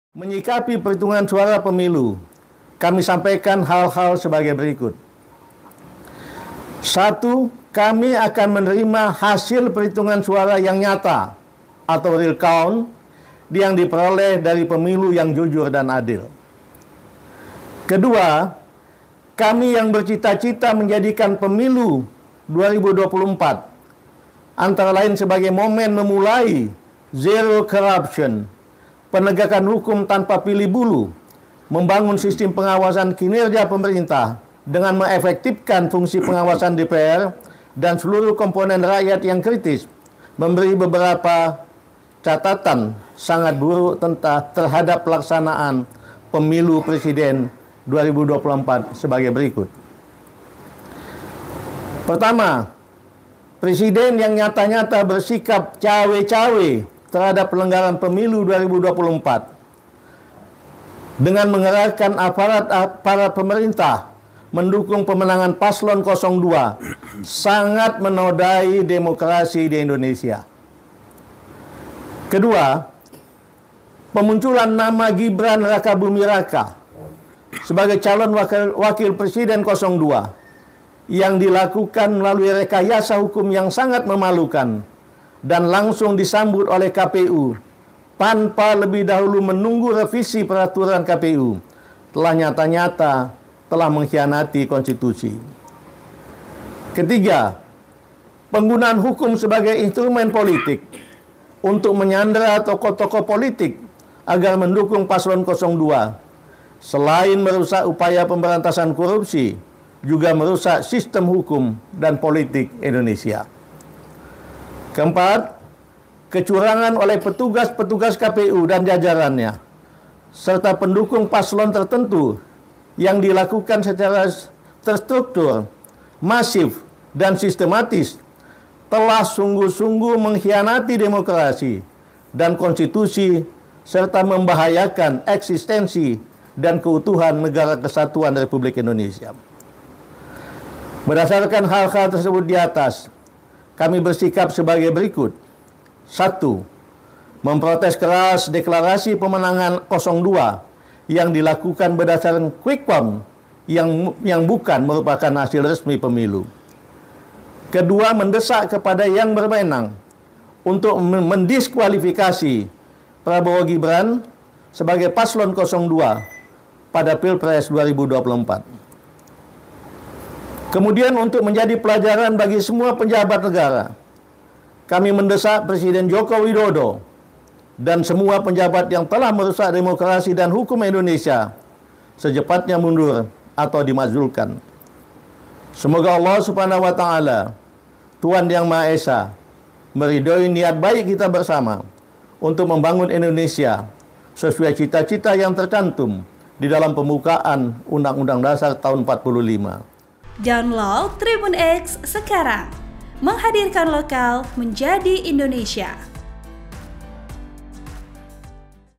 FKP3 Desak Permakzulan Presiden Jokowi Hingga Diskualifikasi Prabowo-Gibran
TRIBUN-VIDEO.COM - Forum Komunikasi Purnawirawan TNI-Polri untuk Perubahan dan Persatuan (FKP3) mendesak pemakzulan Presiden Jokowi.

FKP3 juga meminta pihak berwenang untuk mendiskualifikasi pasangan capres-cawapres Prabowo-Gibran dari Pemilu 2024.